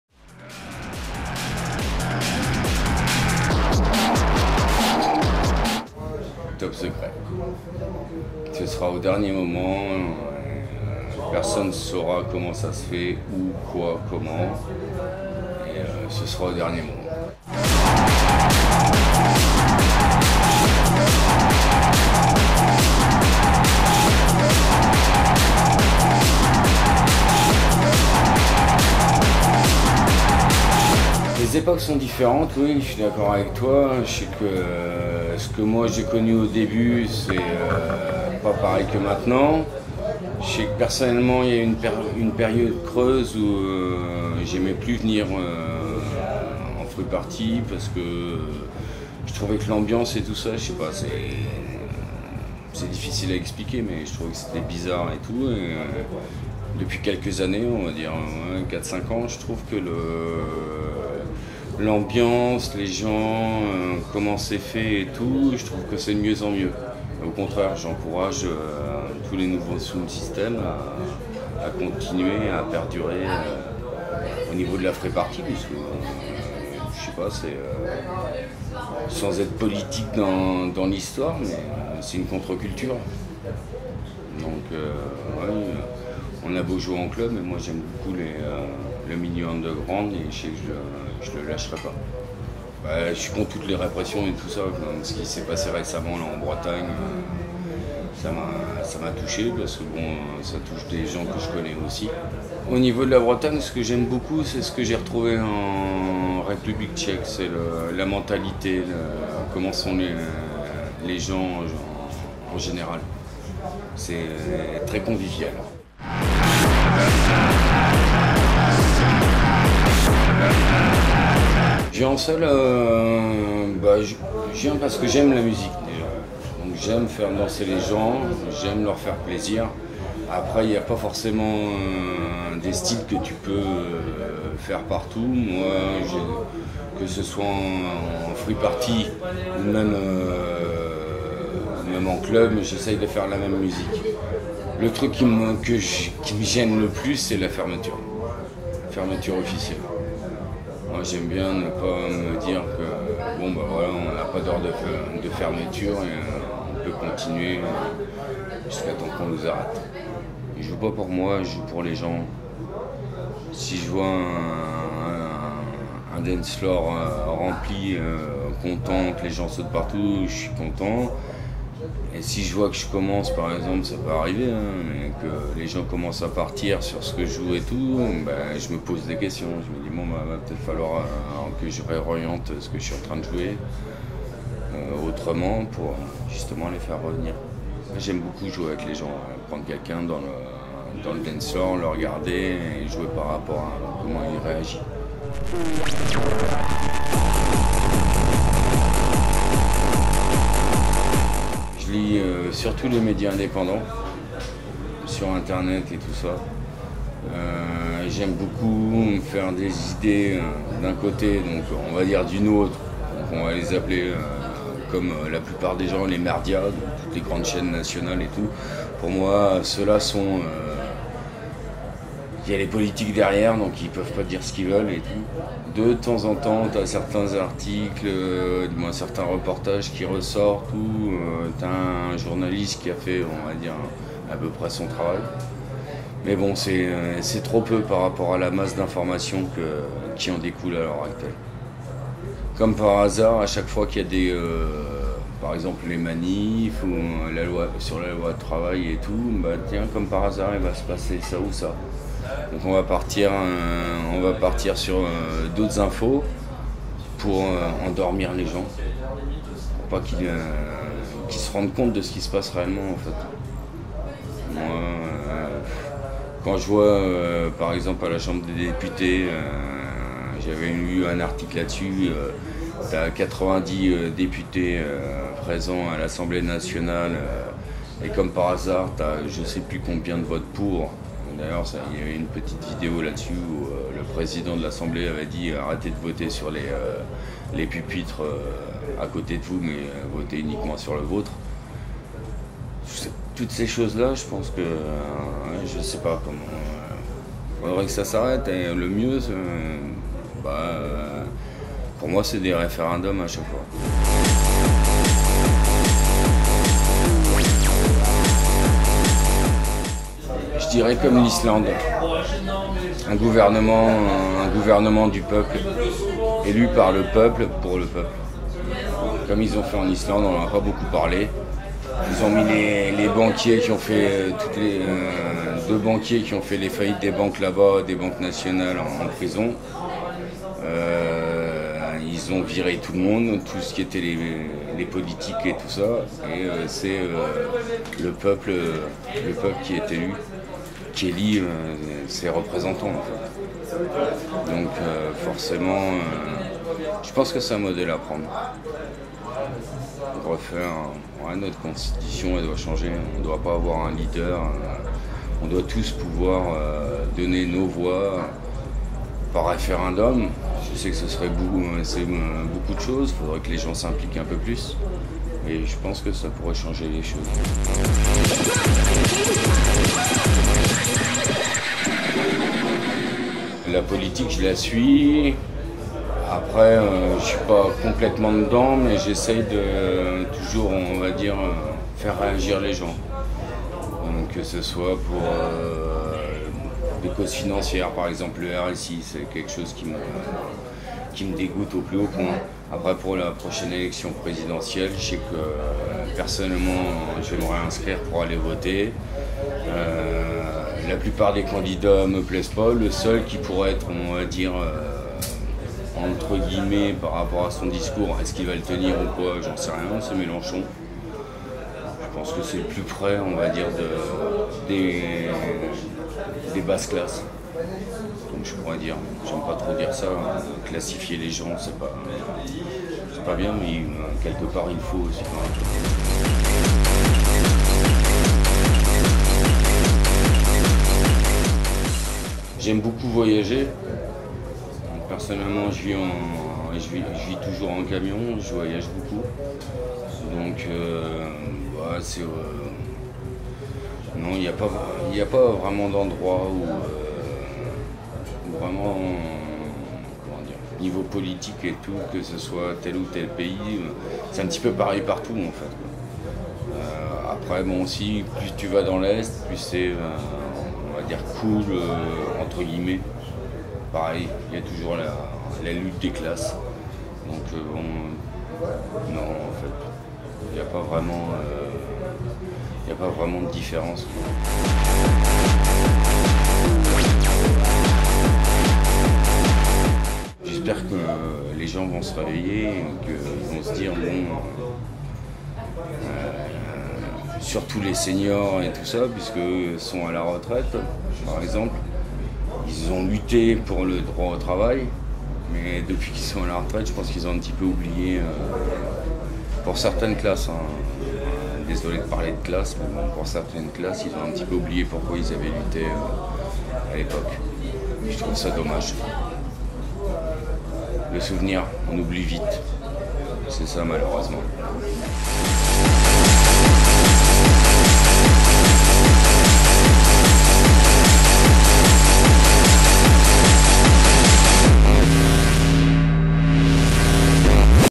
HERETIK - Musik Politik (interview 2016)
Broke du sound-system Heretik nous parle de musique, d'underground et de politique. Connu pour leur free party légendaire à la piscine Molitor en plein Paris, le crew fête ses 20 ans avec une tournée française, en club cette fois-ci.

Interview tourné en 2016 à Rennes au 1988 Live Club.